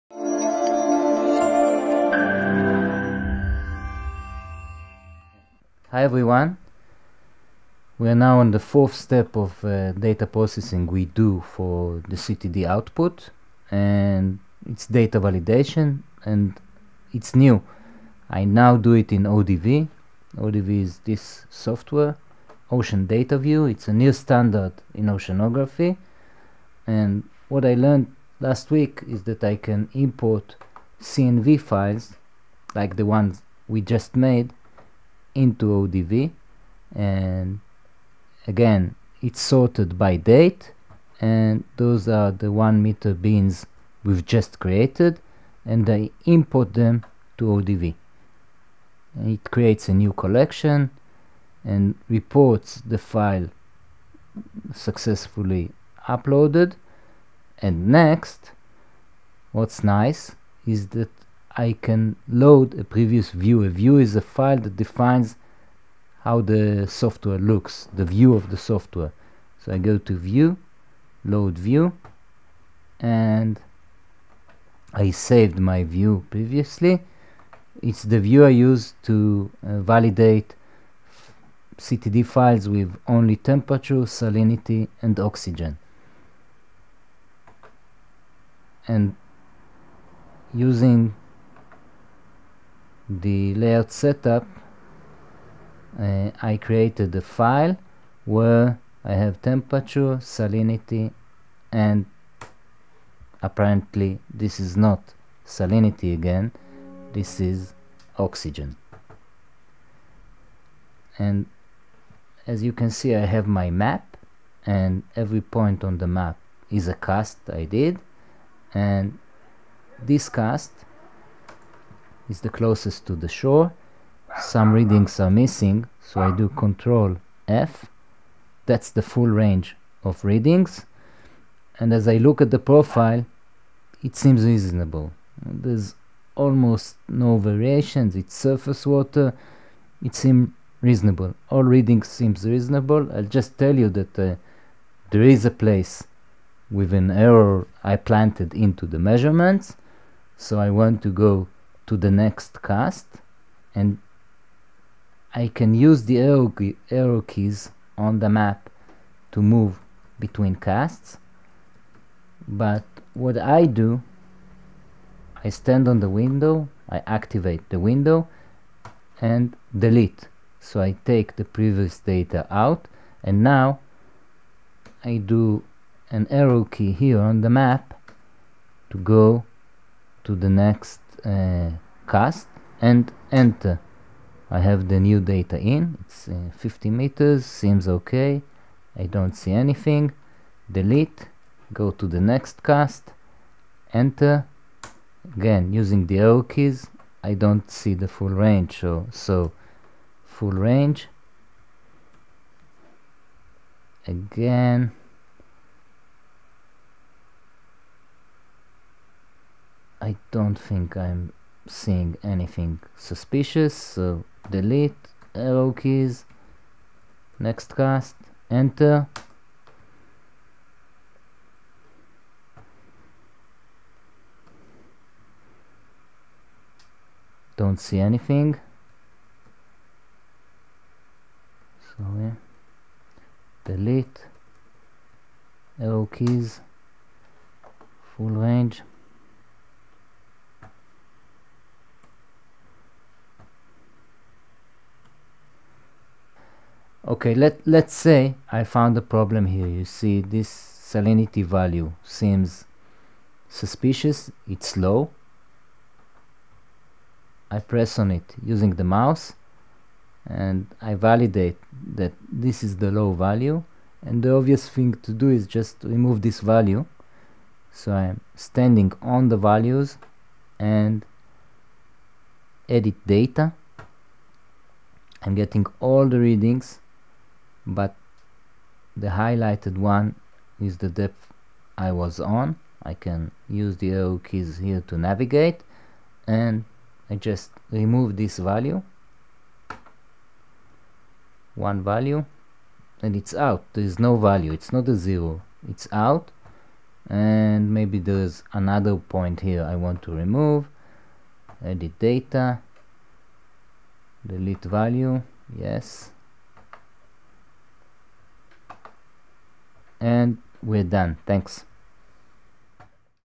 CTD data processing (CTD to ODV) 4 - importing CNV files to ODV and validating the data
How to import CTD multiple cast CNV files to ocean data view and data validation